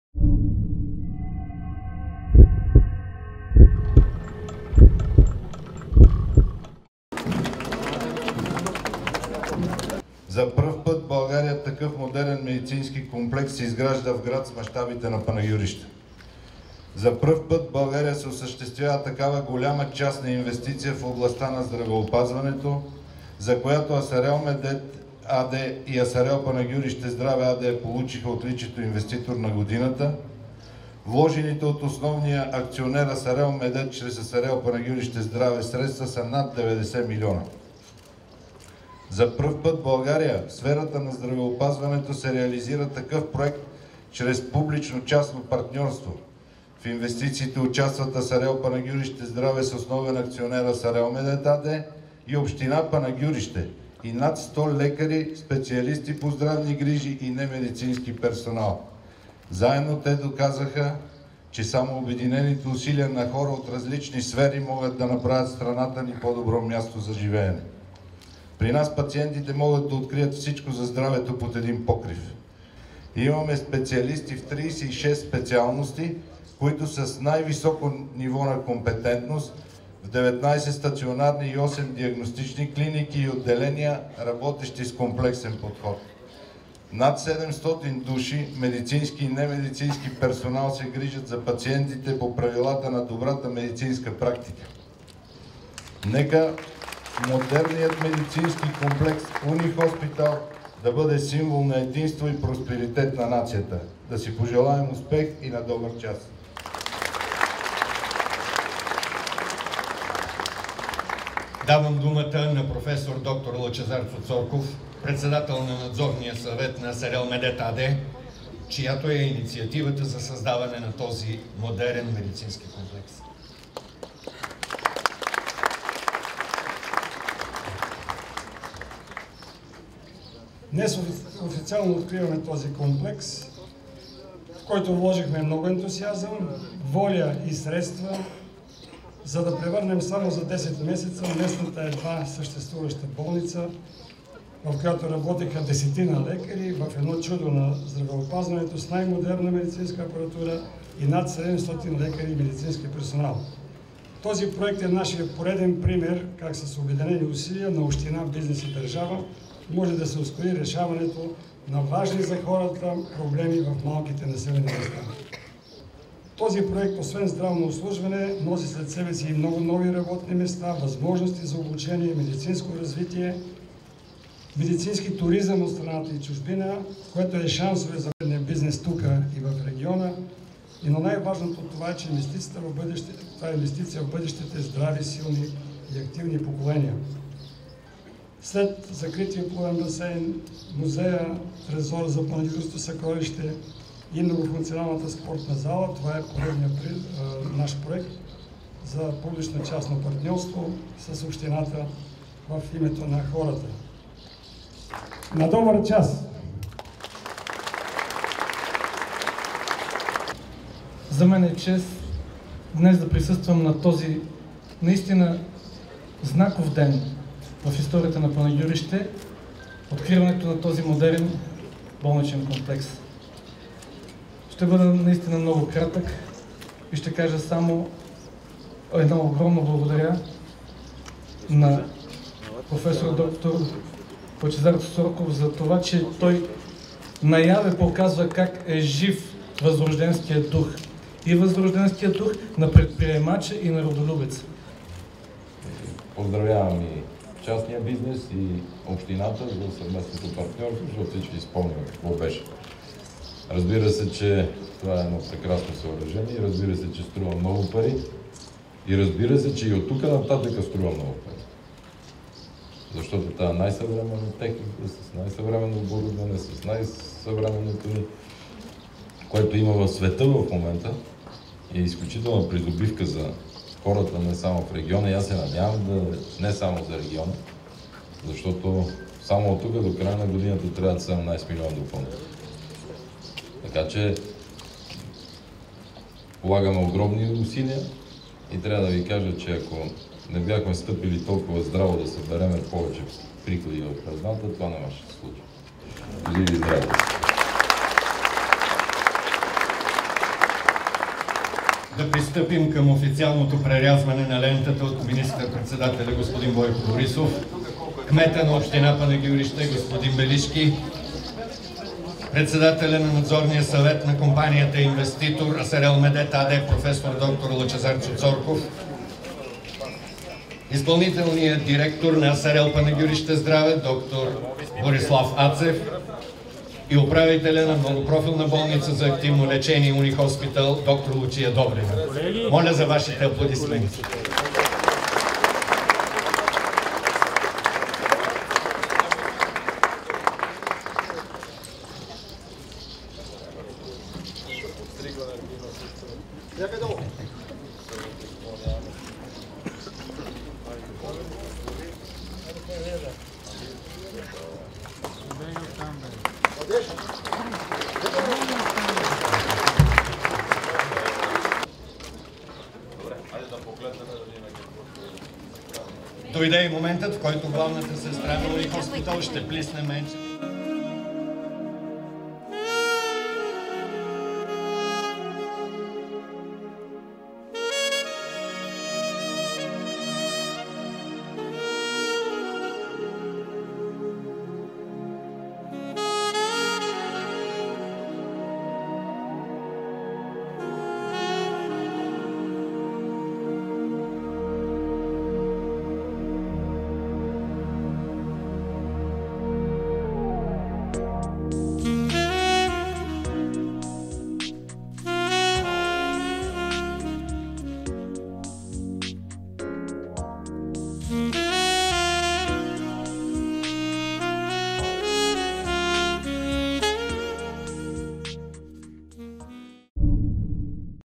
Откриване на новата онкологична болница в Панагюрище
Град Панагюрище вече разполага с високотехнологичен медицински комплекс. Лично министър-председателят Бойко Борисов преряза лентата на откриването на „Уни Хоспитал“ заедно с председателя на надзорния съвет на "Асарел Медет" АД проф. Лъчезар Цоцорков, социалния министър Зорница Русинова, министъра на енергетиката Теменужка Петкова, кмета на Панагюрище Никола Белишки, областния управител на Пазарджик. Вижте атмосфера от събитието и как изглежда клиниката във видеото!